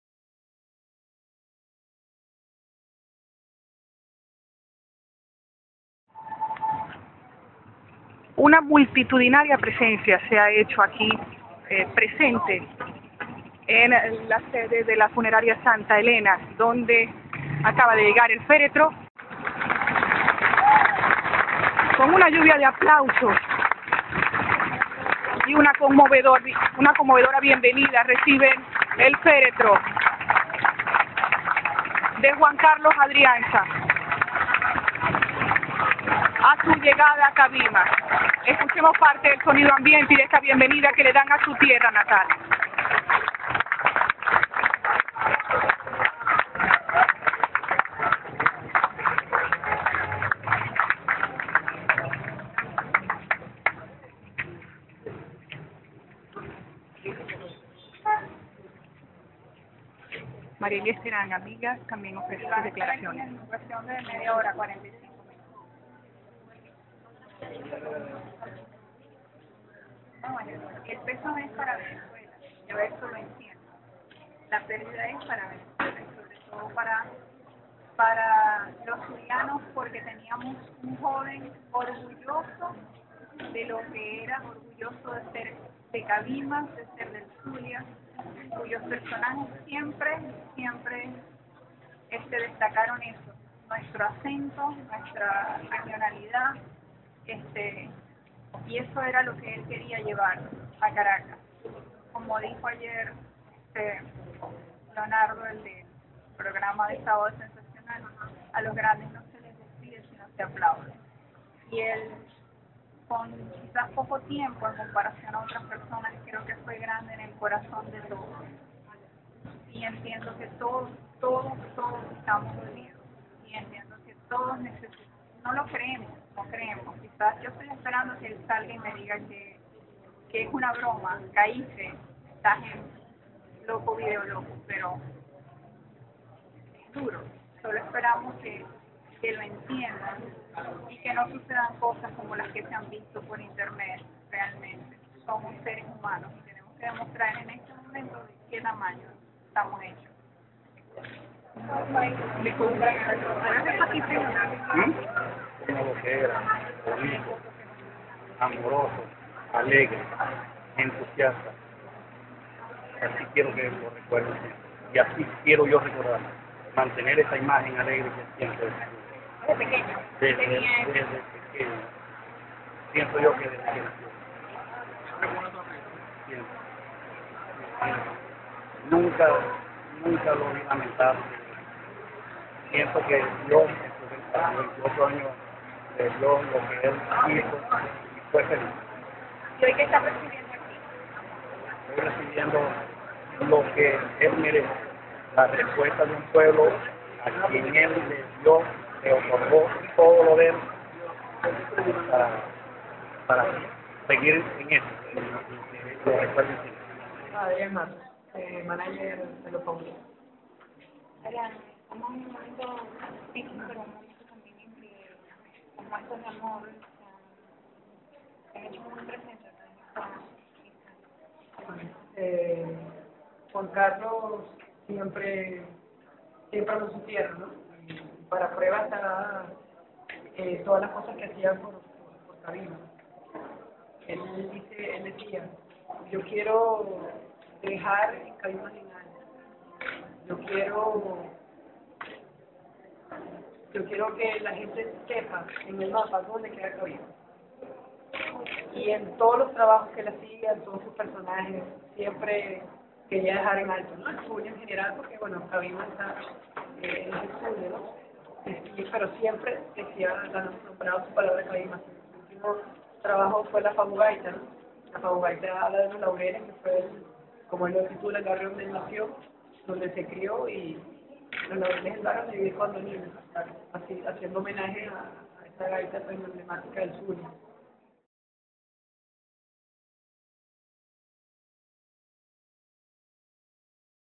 Familia y allegados le dan el último adiós a Juan Carlos Adrianza, el "Fabu Fabu"
A las 9:50 de la mañana de este domingo llegó el féretro con el cuerpo del actor y comediante Juan Carlos Adrianza a Cabimas. Funcionarios de la Policía de Cabimas y de Tránsito Terrestre de la entidad escoltaron la camioneta que transportaba los restos de Adrianza.
Por: Marielys Zambrano